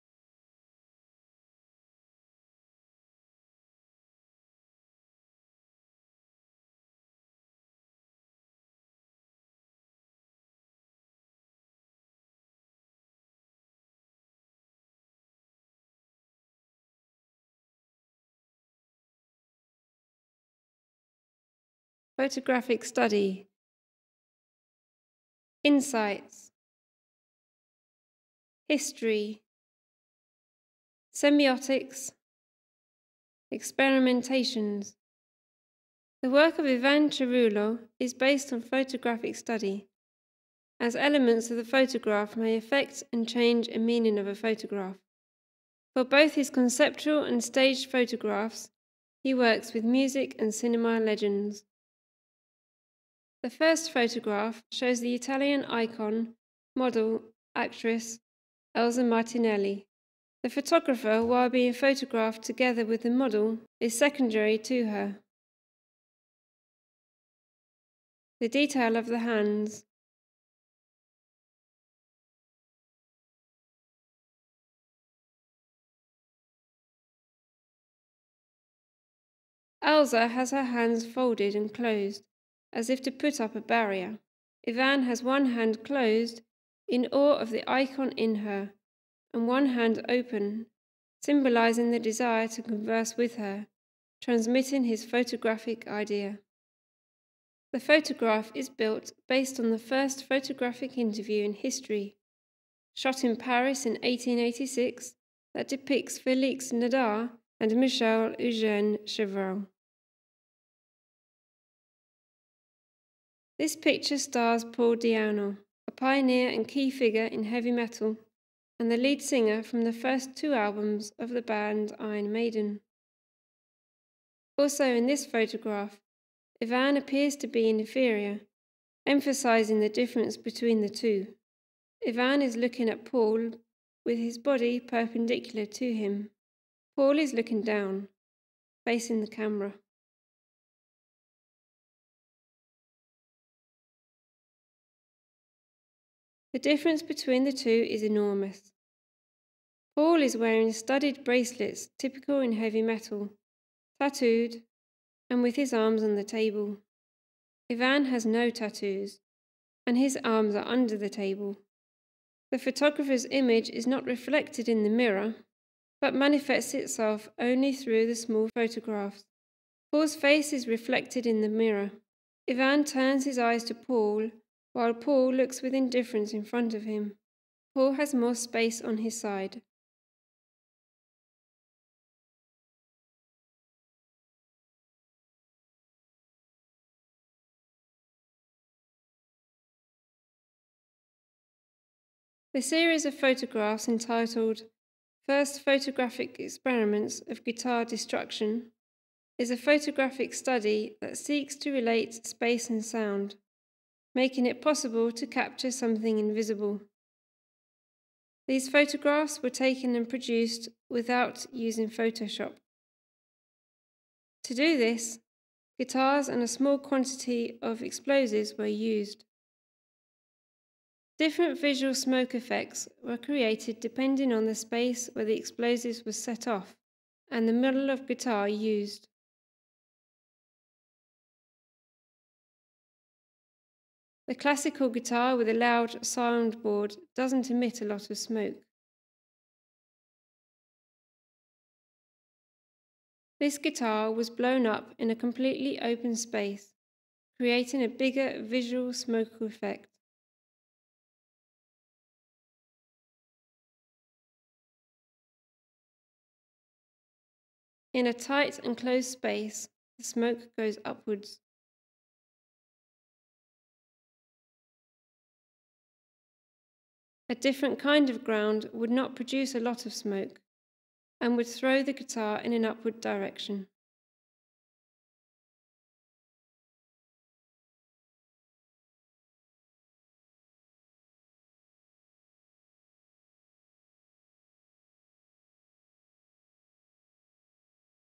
Semiotics on Photography - Ivan Cerullo photographs
Semiotics on Photography - The composition in the photographs of Ivan Cerullo ( Italy b.1979 ) . Staged photography - Conceptual photography .

The video was shot at the " Public photographic archive of Milan " and " Photo laboratory of Milan Politecnico University ".
" Civico Archivio Fotografico di Milano " and " LabFoto Lab Immagine Design Polimi " , Politecnico di Milano .

Special thanks to Dr. Silvia Paoli and Prof. Matteo Bergamini .

Le premier interview photographique. Chevreul. Félix Nadar. Paul Nadar

Semiotics definitions :

_ Is the science that considers the nature of the signs that the mind uses to figure things out or forward our knowledge. ( John Locke )

_ Science that studies the life of signs within the framework of social life . ( Ferdinand de Saussure )

_ Science of the essential nature and fundamental varieties of every possible semiosis,relation to three . ( Charles Sanders Peirce )

_ Discipline that deals with anything that can be used to lie. ( Umberto Eco )

_ The science that encompasses the knowledge of all signs.

_ Roland Barthes argues that signs are not only words but also visual and auditory cues such as gestures, objects, or sounds that can be interpreted within their respective systems of signification ( that is, systematic ways of producing meaning through an interaction with other signs by way of particular, culturally specific patterns ). Semiotics, then, is the study of these signs as they function in language and society.

_ Roland Barthes Codes Theory :

The Hermeneutic Code ( HER )
The Enigma/Proairetic Code ( ACT )
The Symbolic Code ( SYM )
The Cultural Code ( REF )
The Semantic Code ( SEM )

The Hermeneutic Code ( HER ) ( the voice of the truth) Is the way the story avoids telling the truth or revealing all the facts, in order to drop clues in through out to help create mystery.
The Enigma/ Proairetic Code ( ACT ) ( empirical voice) The way the tension is built up and the audience is left guessing what happens next.
The Semantic Code ( SEM ) ( the voice of the person) The semantic code points to any element in a text that suggests a particular, often additional meaning by way of connotation which the story suggests. Connotation= cultural/underlining meaning, what it symbolises.
The Symbolic Code (SYM) ( the voice of symbols) This is very similar to the Semantic Code, but acts at a wider level, organizing semantic meanings into broader and deeper sets of meaning. This is typically done in the use of antithesis, where new meaning arises out of opposing and conflict ideas.
The Cultural Code (REF) ( the voice of science) Looks at the audiences wider cultural knowledge, morality and ideology.